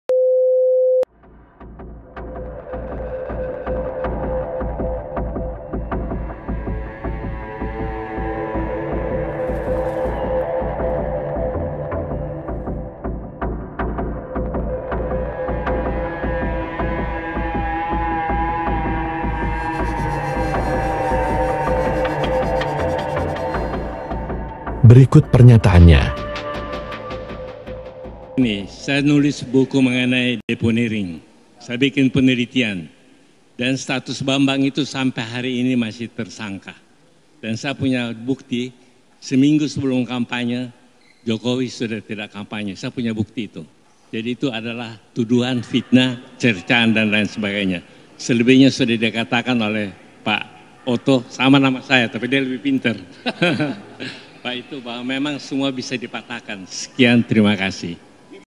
Giliran Kubu Tim Hukum 02 Serang Tim Hukum 01 | OC. KALIGIS : Bambang Masih Berstatus Tersangka
SENGKETA PILPRES || Saling serang antara Tim hukum pasangan calon pun terjadi usai sidang. Saat konfrensi pers, giliran tim hukum 02 Serang Tim hukum 01, kata OC. Kaligis, Bambang masih berstatus tersangka. ikuti liputannya dalam shorts ini